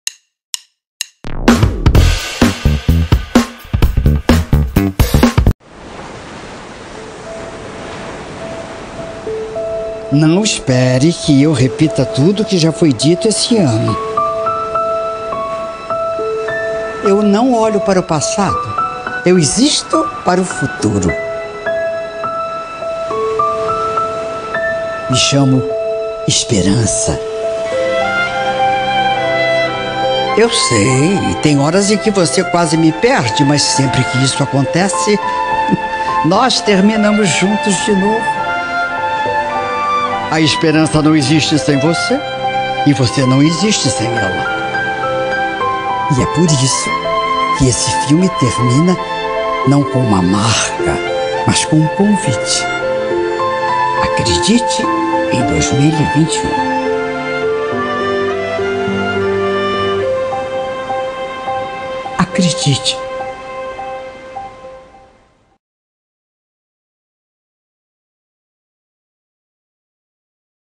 Acredite em 2021
Nesse ano de 2021 todos esperam dias melhores. Também, 2020 não foi fácil para ninguém. Devemos acreditar. Depois da virada, acredite em 2021.